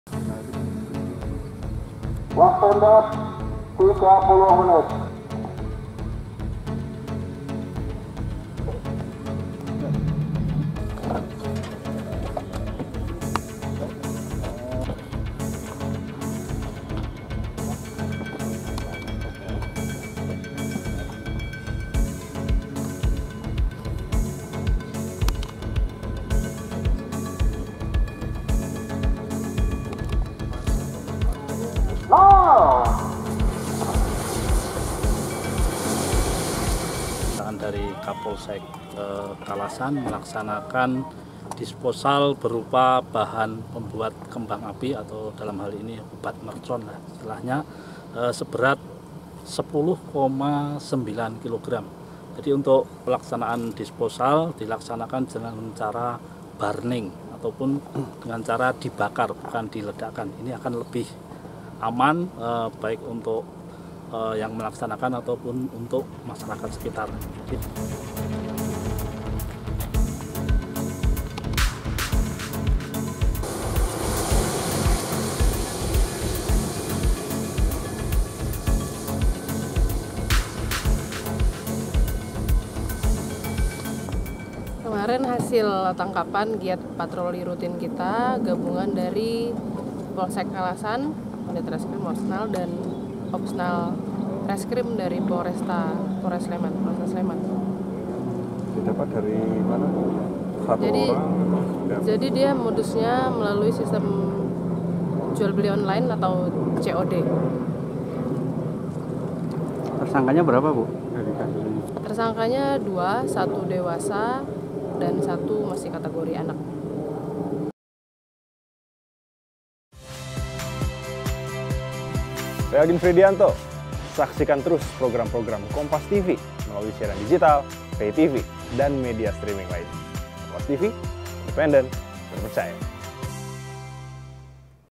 Tim Penjinak Bom Musnahkan Bahan Petasan di Yogyakarta
SLEMAN, KOMPAS.TV   Penjinak bom musnahkan bahan petasan di Yogyakarta.

Tim Penjinak Bom Gegana Polda DIY memusnahkan 11 kilogram bahan petasan. Bubuk petasan tersebut hasil sitaan Polsek Kalasan dari tersangka yang terjaring razia Ramadan.

Pemusnahan dilakukan dengan cara dibakar di lahan kosong yang jauh dari permukiman warga.

Petugas menyebut bahan peledak yang dimusnahkan low eksplosif, bubuk petasan disita dari dua tersangka yakni AR dan AN. Keduanya ditangkap petugas Polsek Kalasan karena kedapatan membawa bubuk petasan saat polisi melaksanakan razia Ramadan.

Baca Juga Operasi Pekat Semeru, Puluhan Kilogram Narkoba dan Ribuan Miras Dimusnahkan di